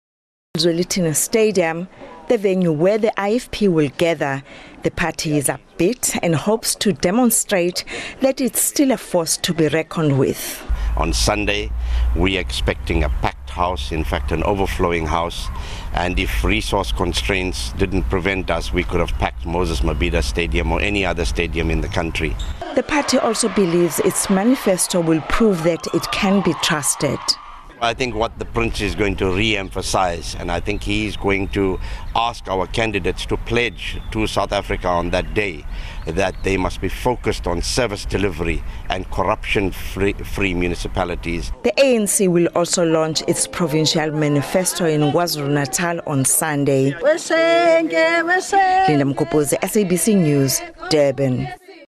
IFP confident that it will improve its performance during elections
The Inkatha Freedom Party says it's confident that it will improve its performance during the local elections. The party's support declined  during the 2014 elections when the  the NFP was established.
The IFP will launch its manifesto over the weekend.